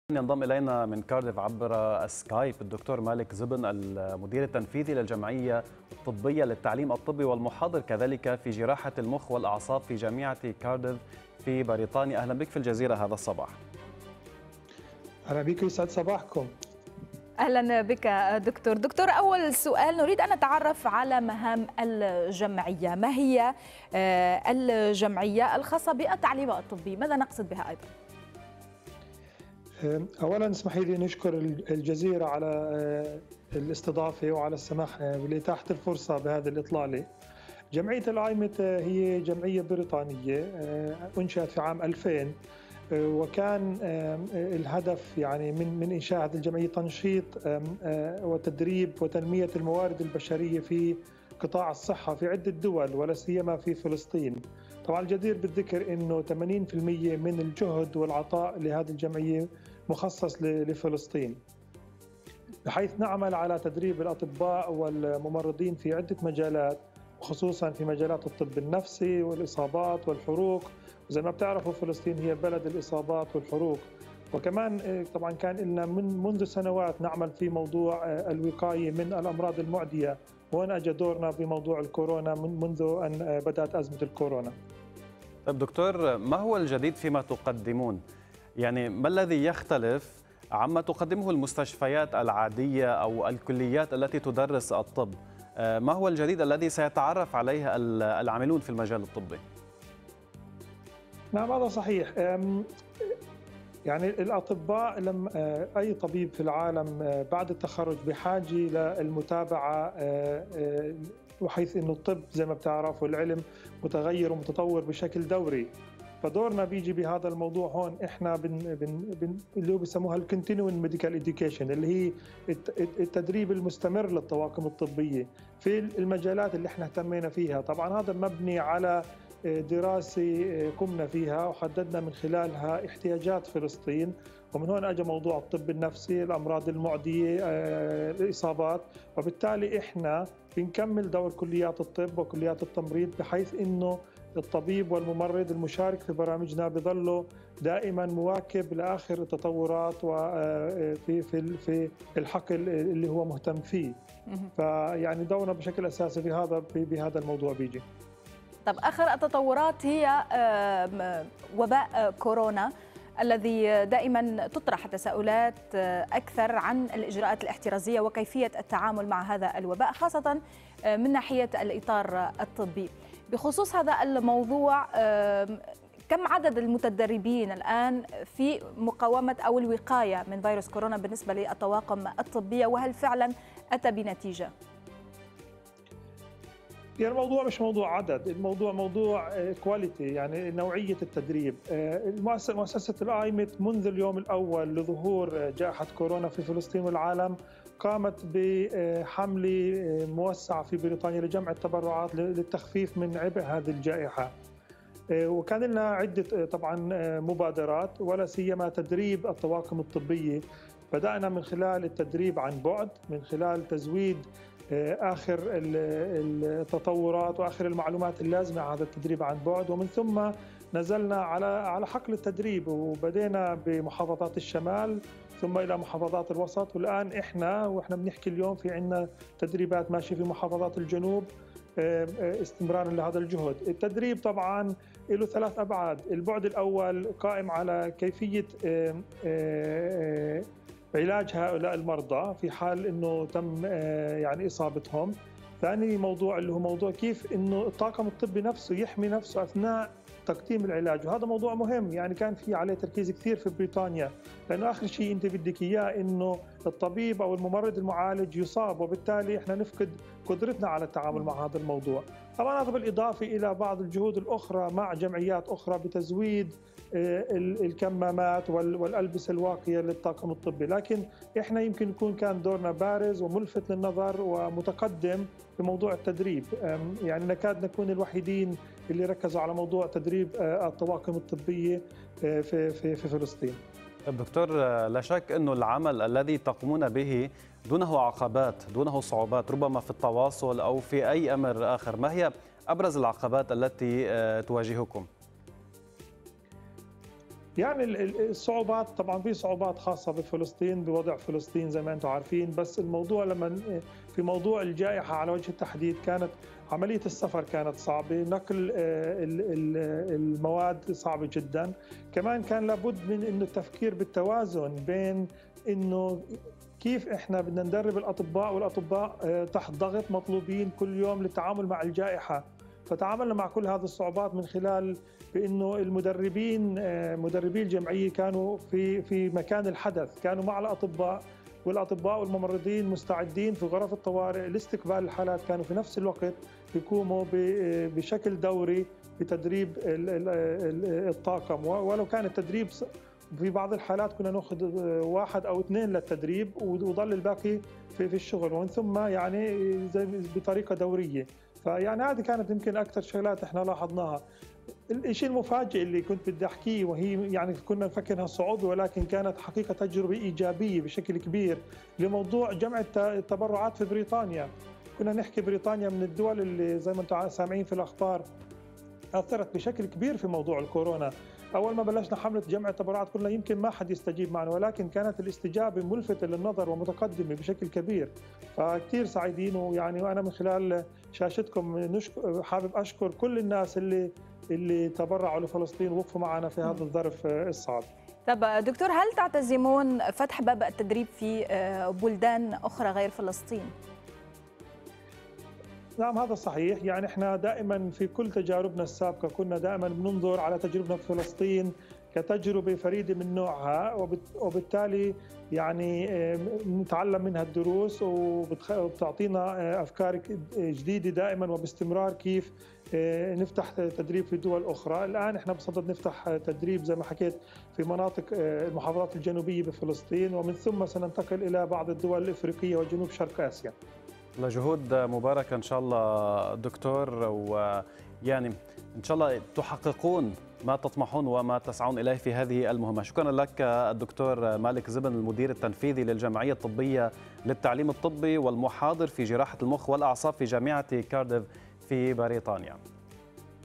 Our CEO's interview on Al-Jazeera
Our CEO Dr Malik Zaben appears on Al-Jazeera to describe IMET2000's activities during the Coronavirus pandemic.